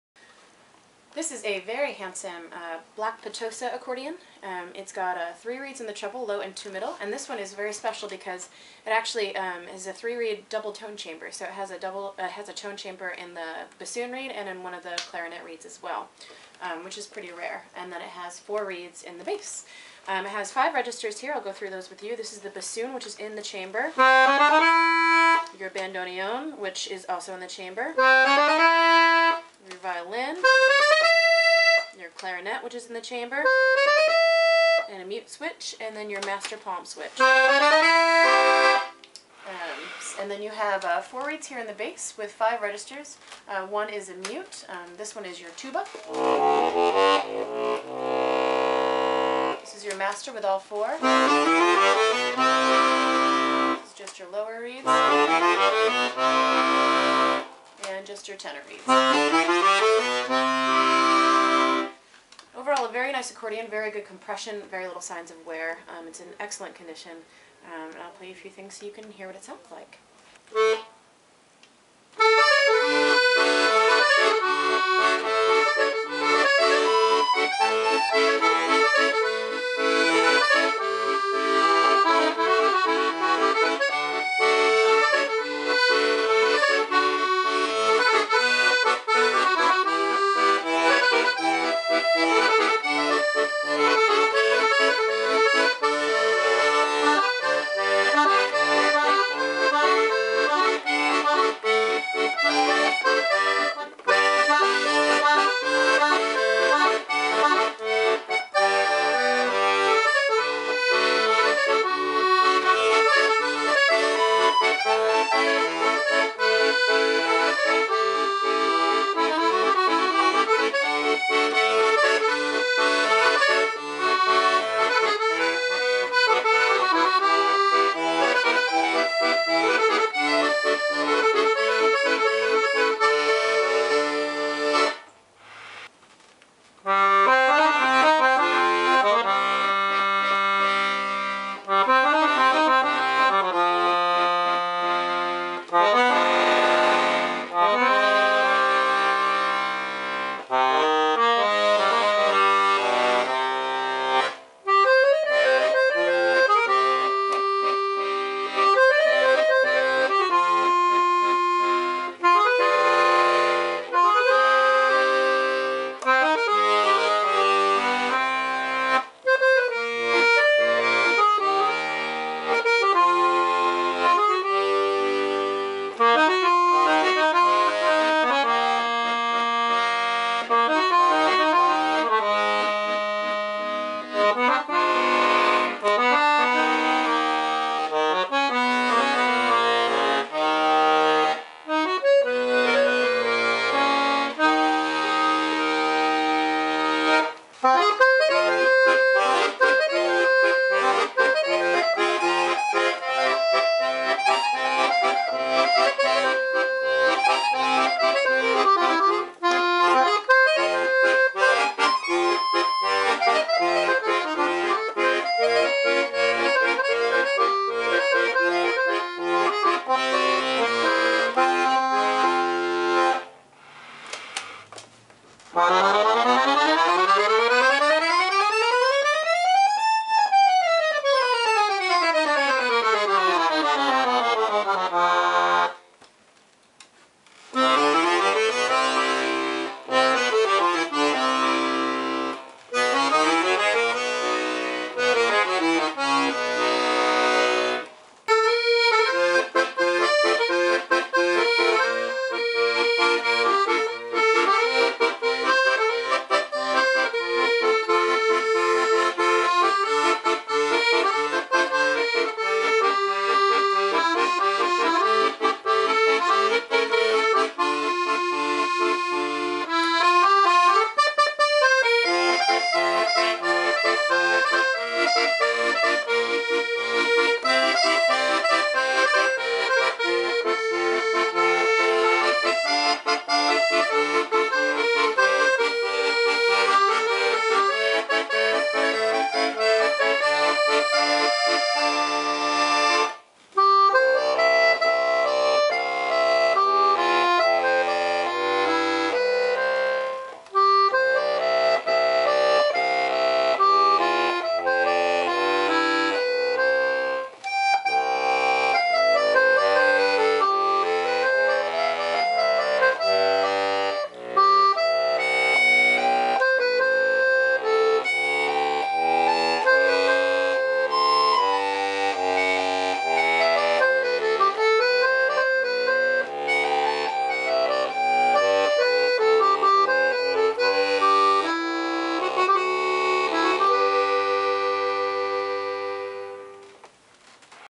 979 - Black Petosa Double Tone Chamber LMM 120 $1495
DETAILS:
Black Petosa (Zero Sette)
Piano Accordion 19.25" 41/120 26lbs
Reeds 3/4 LMM, Registers 5+M/5
Double tone chamber, plus musette reed